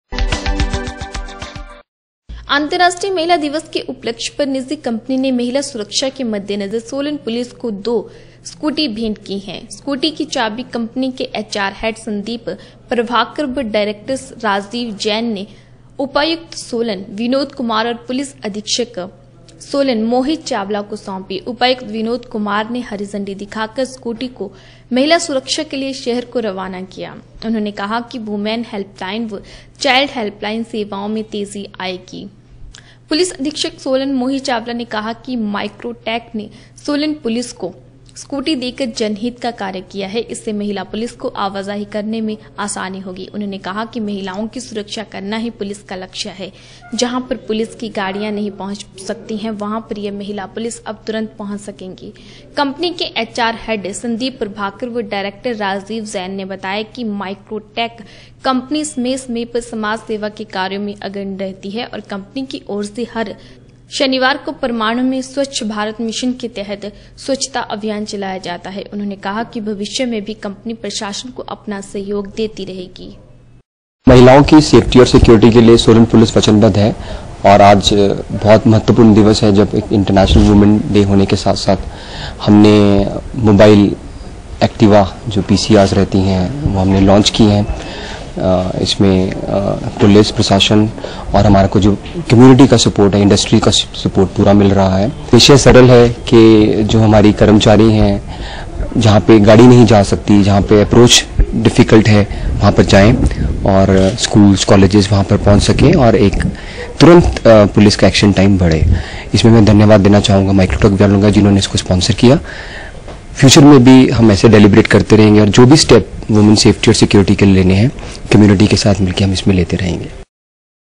निजी कंपनी ने महिला सुरक्षा के मद्देनजर सोलन पुलिस को दो स्कूटी भेंट की गई
अंतर्राष्ट्रीय महिला दिवस के उपलक्ष्य पर  निजी  कंपनी ने महिला सुरक्षा के मद्देनजर सोलन पुलिस को दो स्कूटी भेंट की गई। स्कूटी की चाबियां कंपनी के एचआर हैड संदीप प्रभाकर व डायरेक्टर राजीव जैन ने उपायुक्त सोलन विनोद कुमार और पुलिस अधीक्षक सोलन मोहित चावला को सौंपी। उपायुक्त विनोद कुमार ने हरी झंडी दिखाकर स्कूटी को महिला सुरक्षा के लिए शहर को रवाना किया। उन्होंने कहा कि वूमैन हैल्पलाइन एवं चाइल्ड हैल्पलाइन सेवाओं में तेजी आएगी।